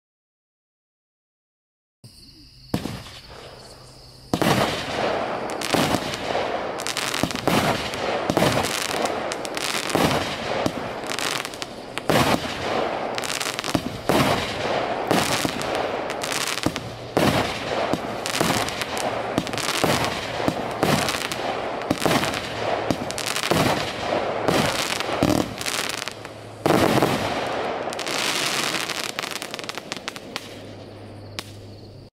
Fiesta
Coloured Tails to red, green, blue, lemon, and purple peonies with crackle

Duration (approximation): 27 sec
Shots: 25